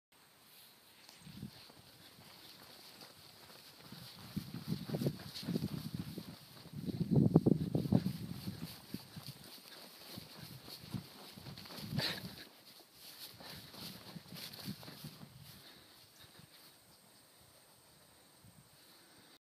Choate Island
Chasing Choate Island wild turkey, summer 2012